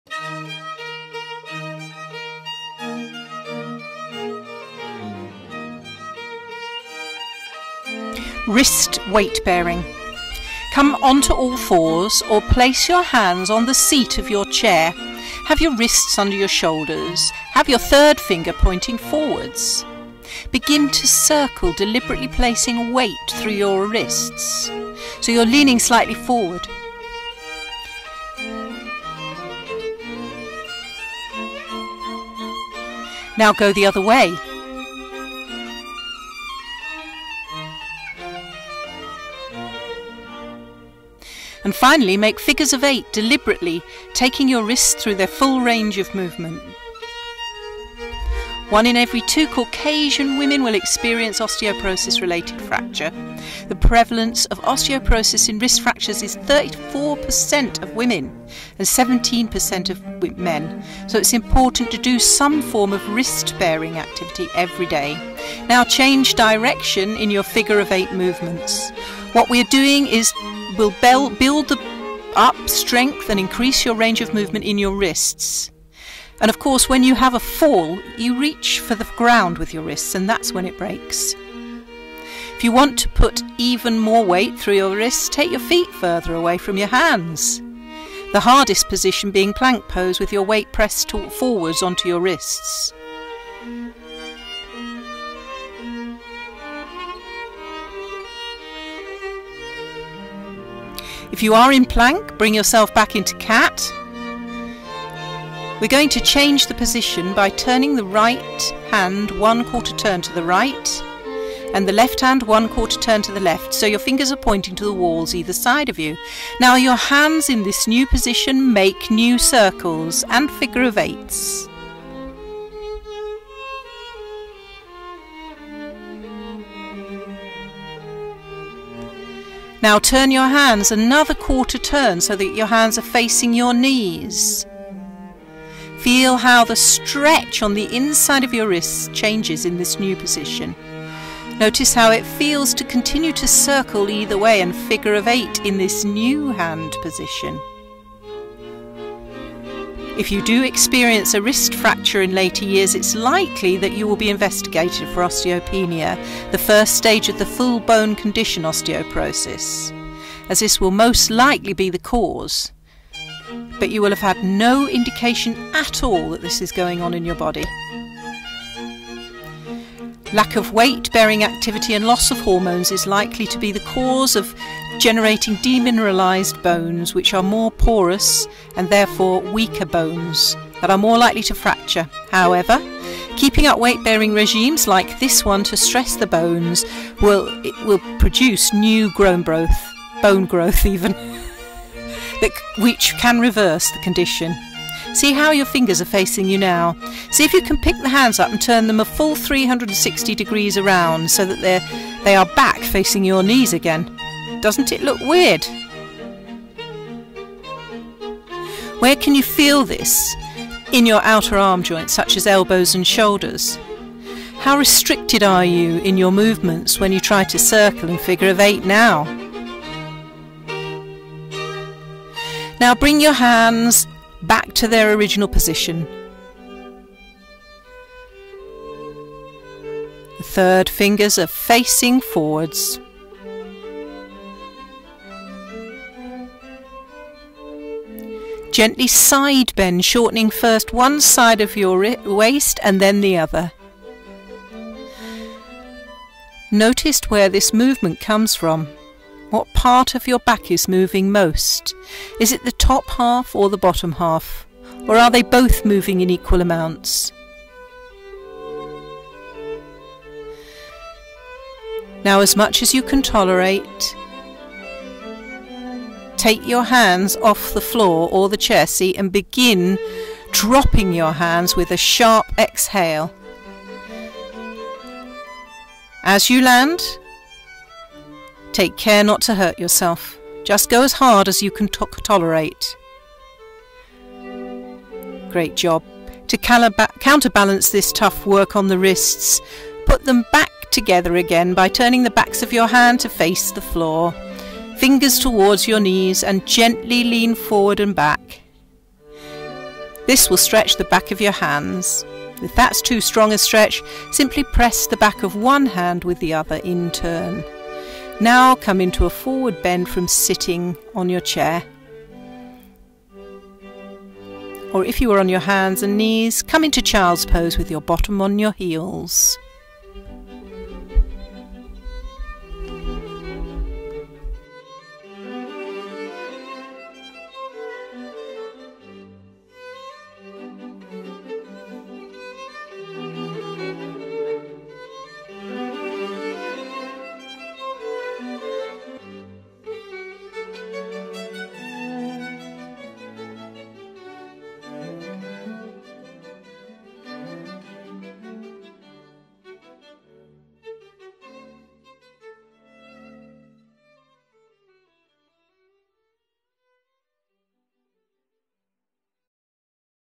Cat Wrist Weight Bearing: Chair Based Yoga Solution for Weak Wrists & prevent Osteoporotic Changes
Here are written instructions for the exercises in this video:

Come on to all fours or place your hands on the seat of your chair.
Have your wrists under your shoulders.  Havre your third finger pointing forwards.
Begin to circle deliberately placing weight through your wrists.
Now go the other way
And finally make figures of 8 deliberately taking your wrists through their full range of movement
One in every 2 causation women will experience an osteoporosis related fraction.
The prevalence of osteoporosis in wrist fractions is 34% of women and 17% of men to it’s important to do some form of wrist weight bearing activity EVERY DAY.
What you are doing will help to build up strength and increase your range of movement in your wrist.  If you want to put even more weight through your wrists, take your feet further away from your hands - the hardest position being plank pose with your weight pressed forwards so that your wrists are taking the strain.
If you are in plank, bring yourself back into cat.  We are going to change the hand position by turning the right hand one quarter turn right, left hand one quarter turn left so your fingers are pointing to the walls either side of you.  Now with your hands in this new position:
Now make circles
And figures of 8’s.
Now turn your hands another 1/4 turn so that your hands are facing your knees.  Feel how the stretch on the inside of your wrists changes in this new position.  Notice how it feels to continue to circle either way and figure of 8 with this new hand position.
If you do experience a wrist fracture in later years, it is likely that you will be investigate for Osteopaenia - the first stages of the full blown condition of Osteoporosis - as this will most likely the the cause but you will have had no indication at all that this is going on in your body.  Lack of weight bearing activity and loss of hormones is likely to be the cause of generating de-mineralised bones which are more porous and therefore weaker bones that are more likely to fracture however, keeping up a weight bearing regime to stress the bones into producing new bone growth can reverse the condition.

See how your fingers are facing your knees now...  see if you can pick the hands up and turn them a full 360 degrees around so they are back facing our knees again.  Doesn’t it look weird?  Where can you feel this in your other arm joints such as elbows and shoulders?  How restricted are your movements when you try to circle and figure of 8 now?

Now bring your hands back to their original position, third finger facing forward.  Gently side bend, shortening first one side of your waist and then the other.  Notice where this movement comes from.  What part of your back is moving most?  The top half or the bottom half or are they both moving in equal amounts?

Now, as much as you can tolerate, take your hands off the floor or the chair seat and begin dropping onto your hands with a sharp exhale as you land, as hard as you can tolerate, taking care not to hurt yourself...

To counterbalance this tough work on the wrists and put them back together again, turn the backs of your hands to face the floor, fingers toward your knees and gently lean forward and back.  This will stretch the back of your hands.  If that is too strong a stretch, simply press the back of one hand with the other in turn.  Now come into a forward bend from sitting on your chair or if you were on hands and knees, come into child’s pose with your bottom on your heels.

In either of these positions... press the back of one hand with the other, palm coming toward the inside of your lower arm bone.  Switch sides.
Circle them round and round until you feel they are fully recovered from taking all your weight.